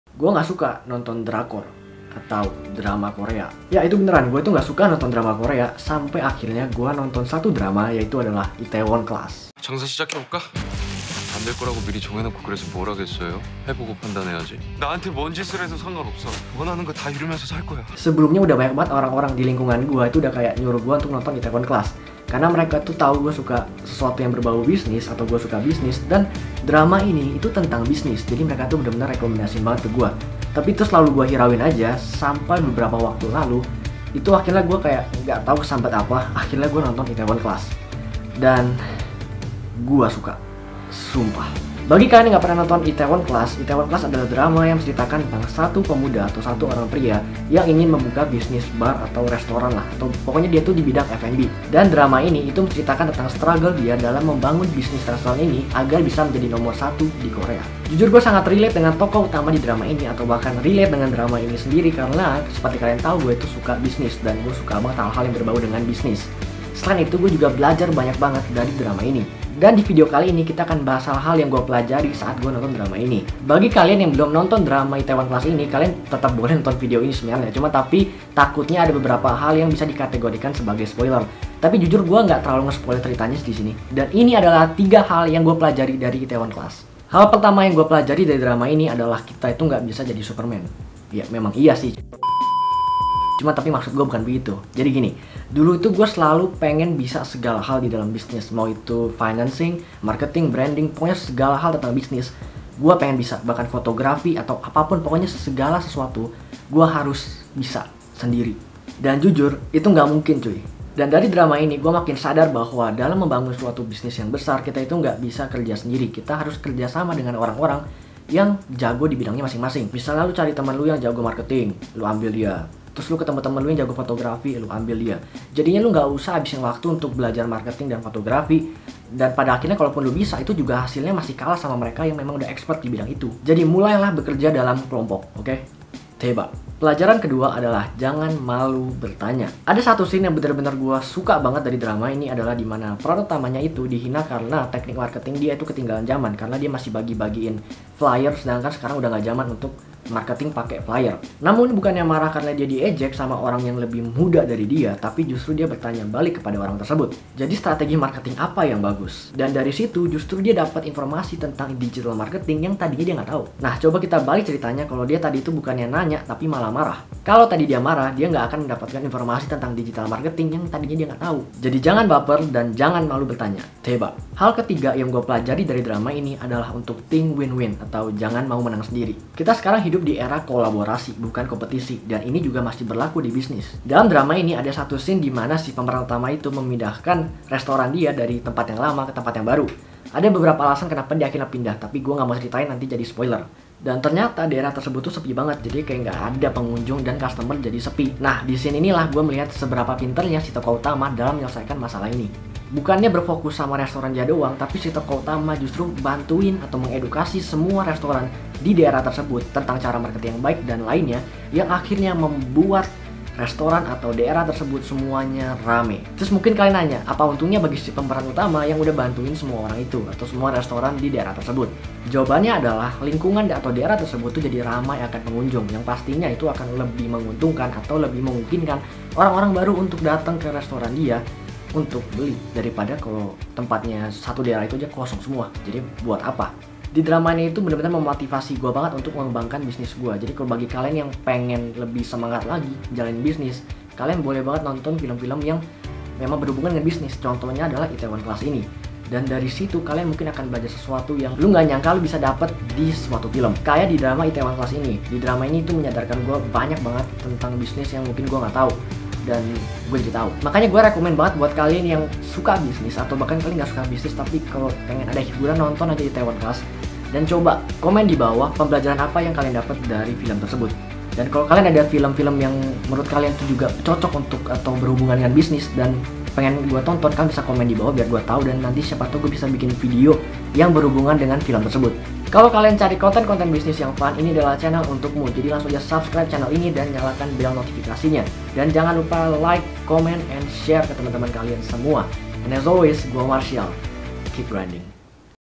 HAL YANG GUA PELAJARI DARI ITAEWON CLASS !! BELAJAR BISNIS DARI DRAMA KOREA ??
Yo guys di video kali ini, gua bakal bahas 1 drama korea yang gua suka banget nih, namanya itaewom class. Gua belajar banyak banget dari drama ini jadi di video kali ini gua bakal bahas hal yang gua dapet dari drama ini !!